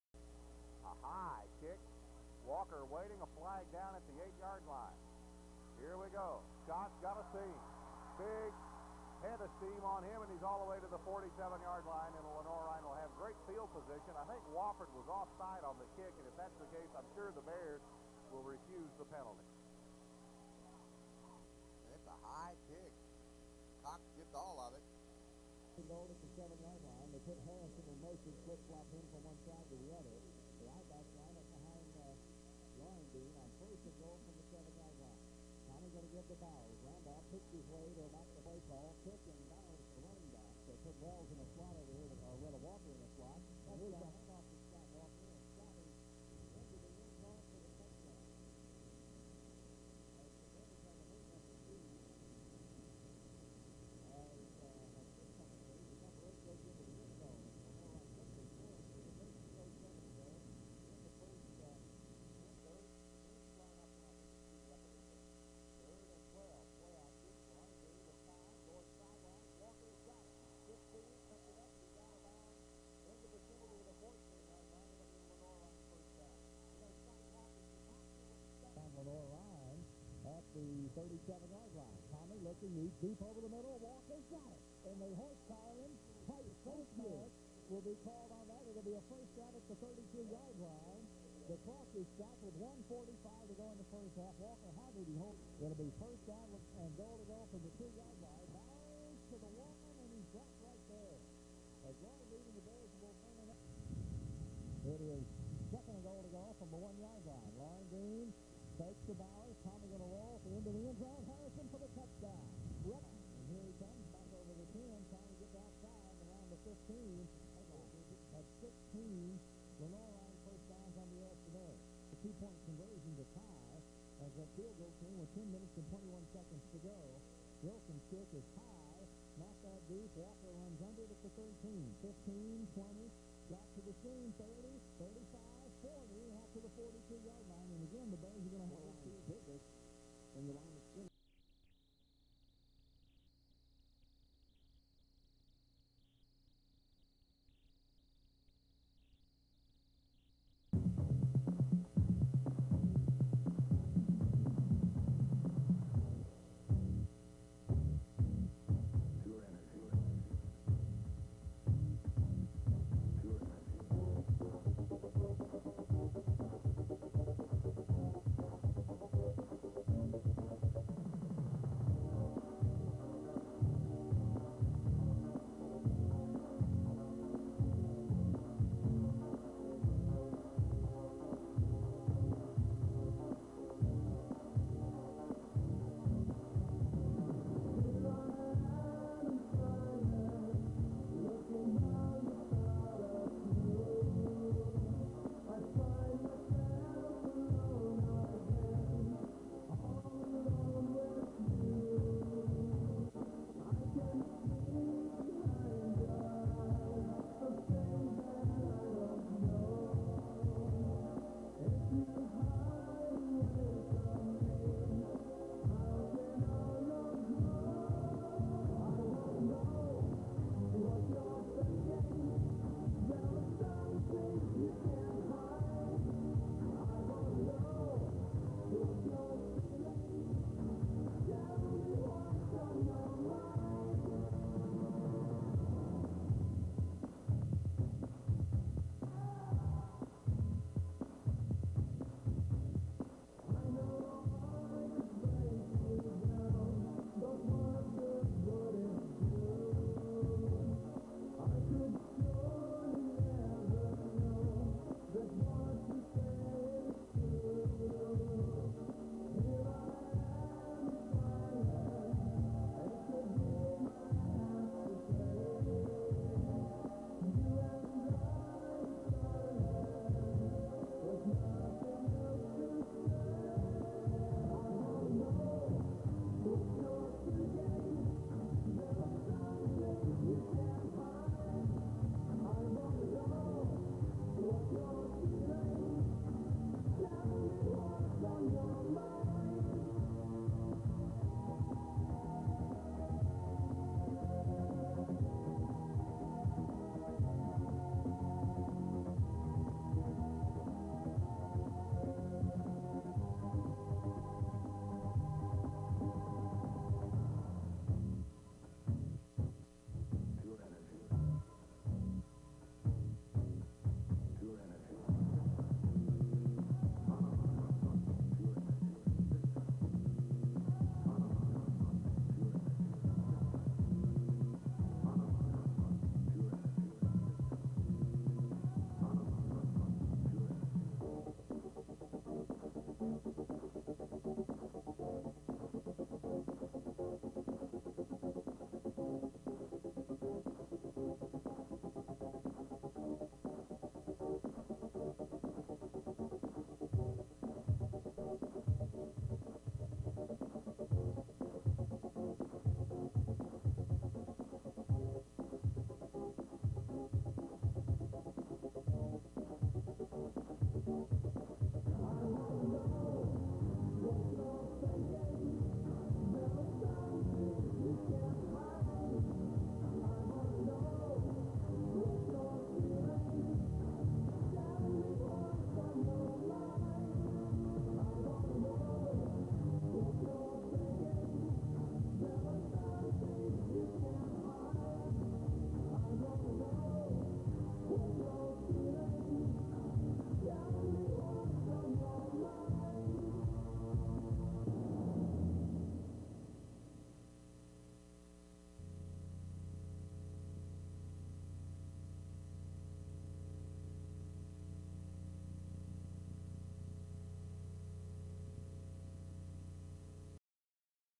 LR 1988-89 SAC championship season.
Various Highlights of  Lenoir Rhyne College's 1988 SAC championship season.